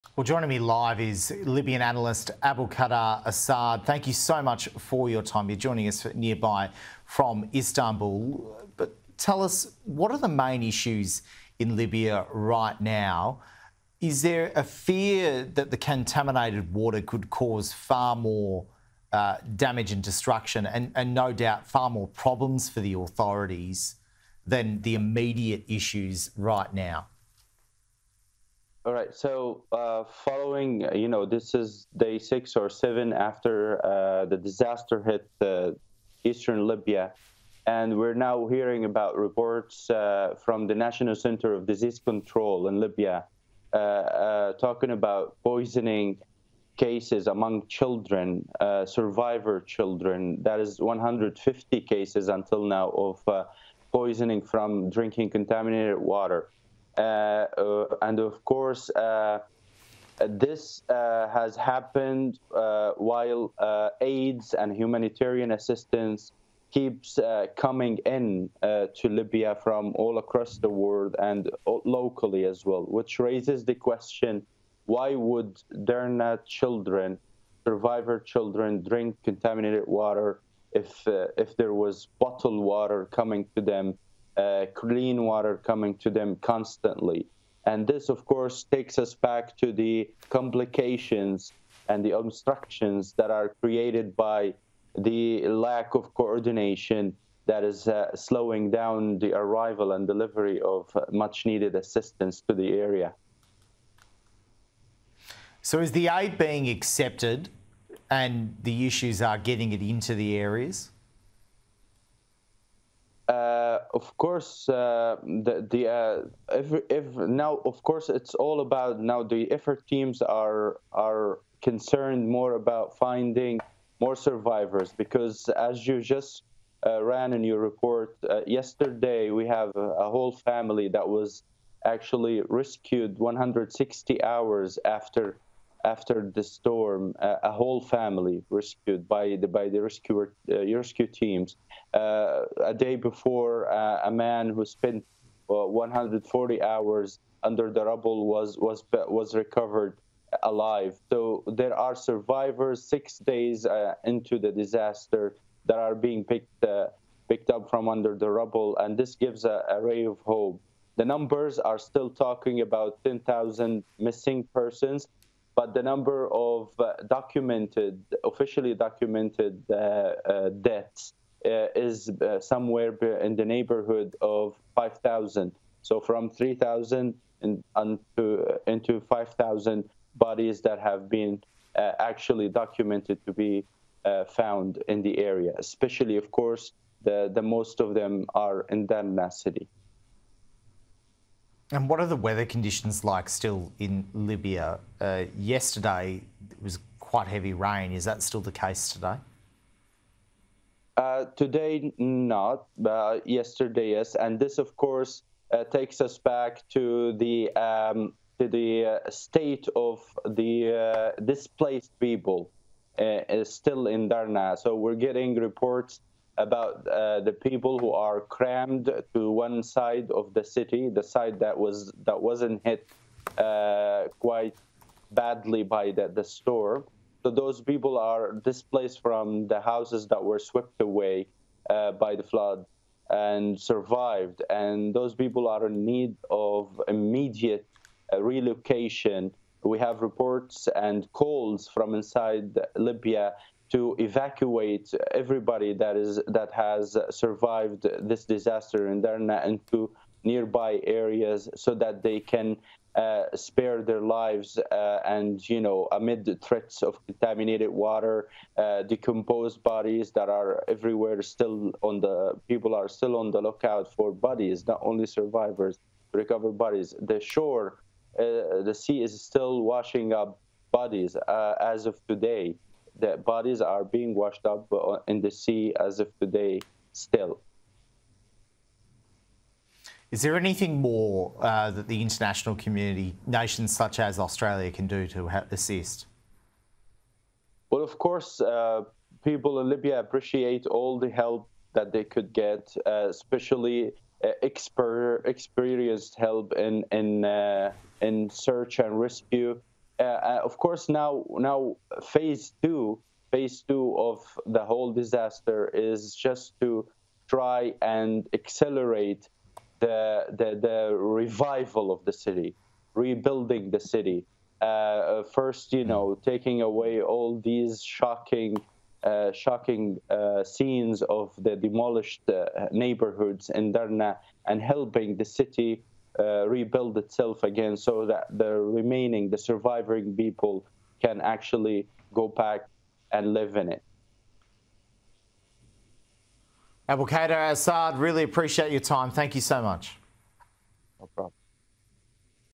Questions raised over why children in Libya are choosing to drink contaminated water
Questions have been raised over why children in Libya are choosing to drink contaminated water despite humanitarian aid being sent to the country, according to political analyst Abdulkader Assad.

Up to 3,000 people have died with 10,000 missing after Mediterranean Storm Daniel triggered flash flooding across eastern Libya.

“This has happened while aids and humanitarian assistance keeps coming in to Libya from all across the world and locally as well," he told Sky News Australia.

“Which raises the question, why would Derna children – survivor children drink contaminated water if there was bottled water coming to them, clean water coming to them constantly.

“This of course takes us back to the complications and the obstructions that are created by the lack of coordination that is slowing down the arrival and delivery of much needed assistance to the area.”